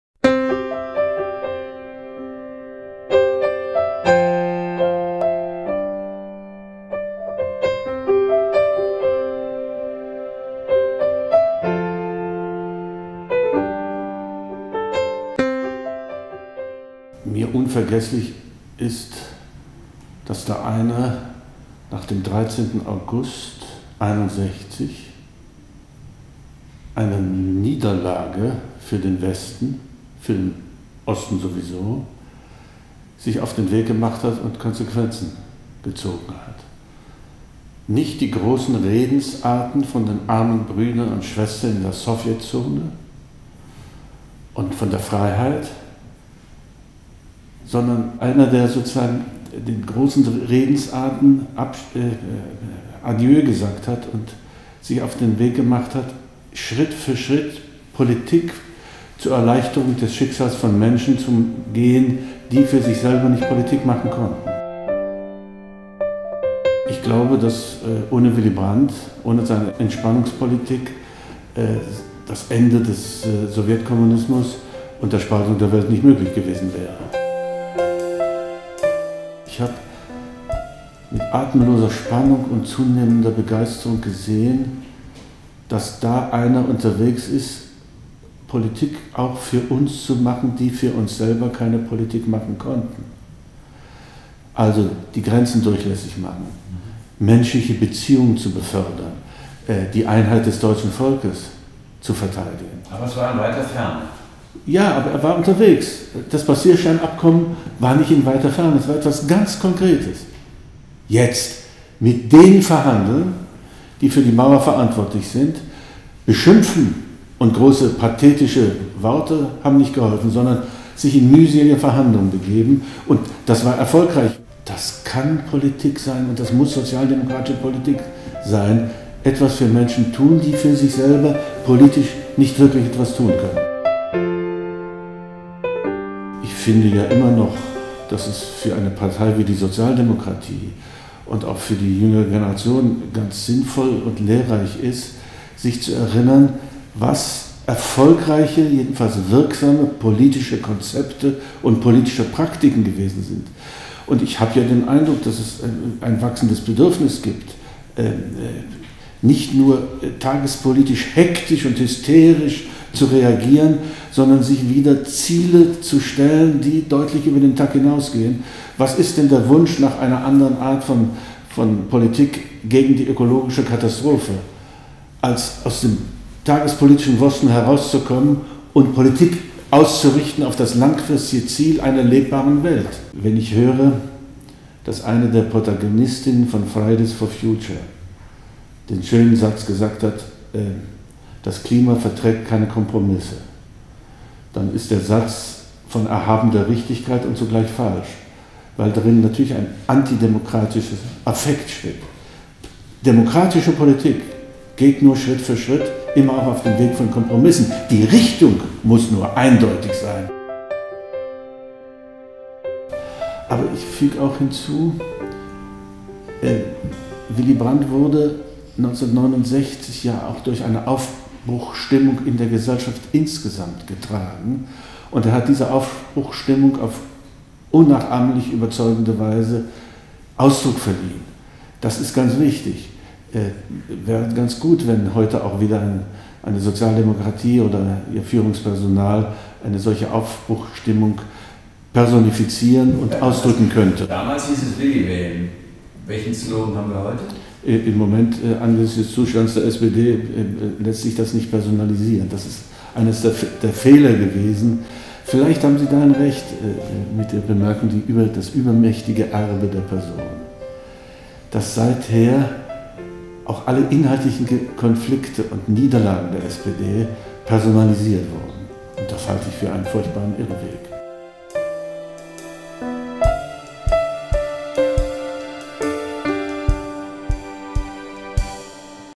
Wolfgang Thierse – 50 Jahre Kanzlerschaft Willy Brandt (Interview)
Wolfgang Thierse – 50 Jahre Kanzlerschaft Willy Brandt: Der Kuratoriumsvorsitzende der Bundeskanzler-Willy-Brandt-Stiftung, Wolfgang Thierse, im Gespräch mit Stephan-Andreas Casdorff. Ein gemeinsames Projekt von  @DerTagesspiegel und der Bundeskanzler-Willy-Brandt-Stiftung.